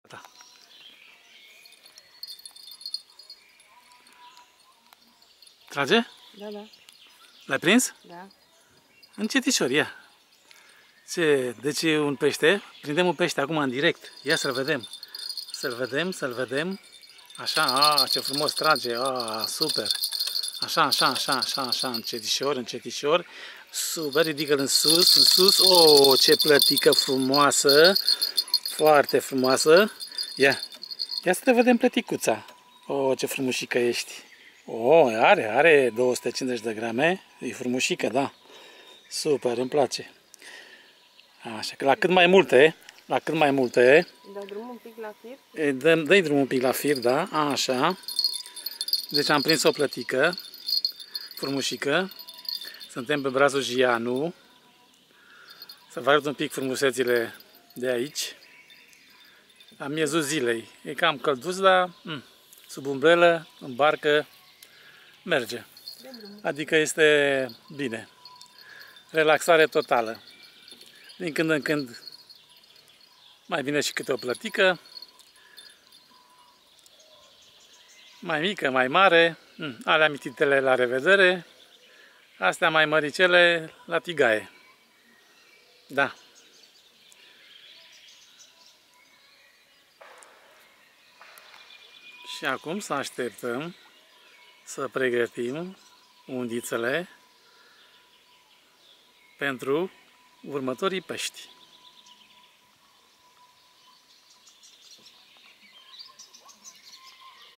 pe Ostroavele Dunării (2)@Pescuit și relaxare pe bratele secundare ale Dunarii
Călătorii pe Ostroavele Dunării (2)
Pescuit și relaxare pe Brațul Jianu

Travels on the Danube Islands (2)
Fishing and relaxing on Jianu Arm

.momente de liniște și bucurie pe brațul Jianu,
departe de betoane și asfalt.
...liberi ca păsările ce-şi scutură din aripi albastrul cerului ,
liberi  ca peștii plescăind  pe sub sălcii,
 prin cotloane  ascunse.